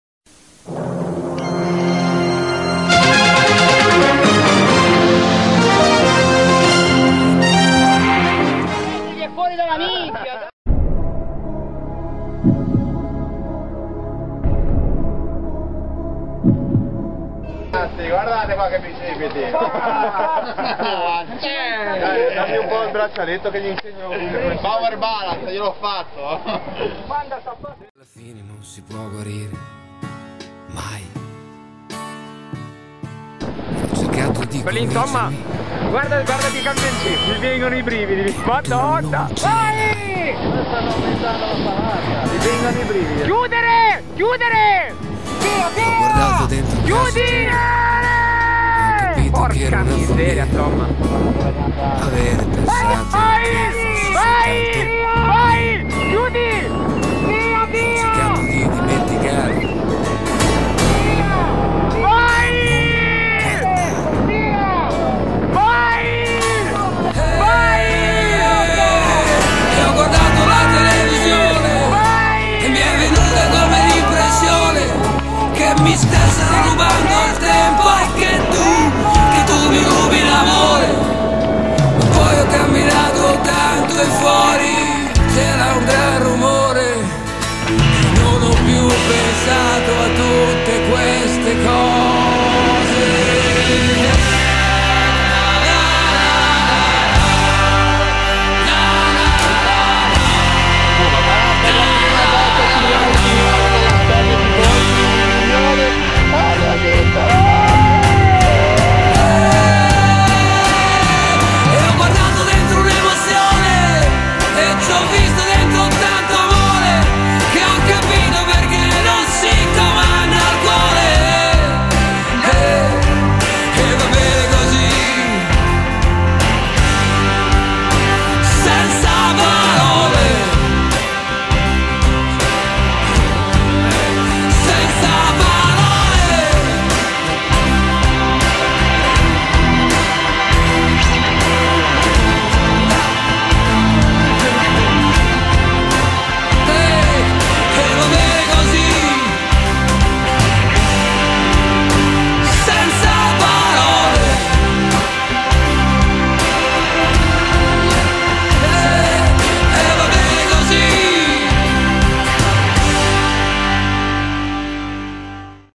Palio del Levante celebrazione.wmv
Canottaggio.
Celebrazione della prima vittoria di sempre, per un gozzo categoria juniores, della Canottieri Voltri